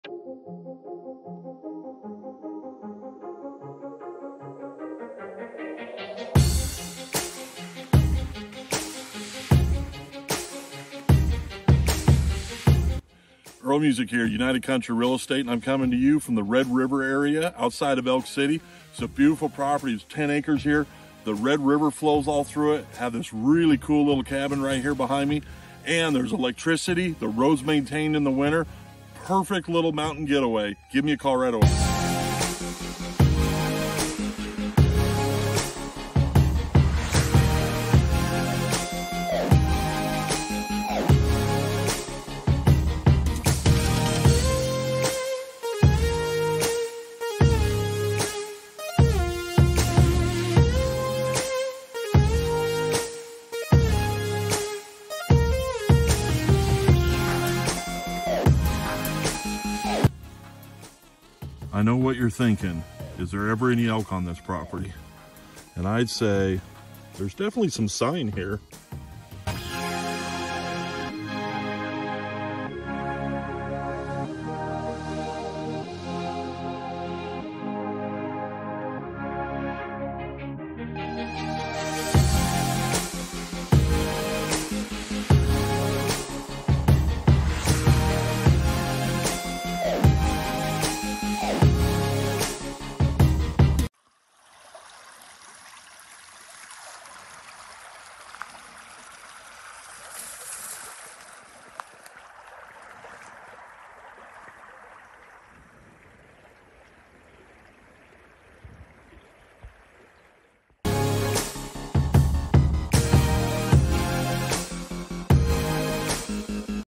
SOLD! Idaho Log Cabin for sale on the Red River Near Dixie SOLD!
SOLD! But we have more
Welcome to @idahorealestate your North Central Idaho real estate resource.
Realtor Earl Musick
Address 2266 Red River Rd Elk City, Idaho
MLS# 98863082
Price $345,000 SOLD! but we have more.
Perfect Cabin Getaway at the gateway to the Frank Church wilderness and boarding the Nez Perce National Forest.  This 10-acre property is fenced for horses or mules. This is the last cabin on the paved road with power, septic, and spring.  Bring your snow machines and UTV for great trails, fishing high mountain lakes, and hunting in the backcountry.

The small cabin is one room with a kitchen,  bathroom, and wood stove for heat. Enjoy the mountain air on the small covered decks off the front and back.

The Red River hosts Chinook Salmon in the spring and fall spawning grounds. You will see the Elk and Black bear wander through the meadows.

The cabin is 11 miles from the Red River Hot Springs which hosts hot tubs and a swimming pool with cabins and hotel rooms for guests to stay over.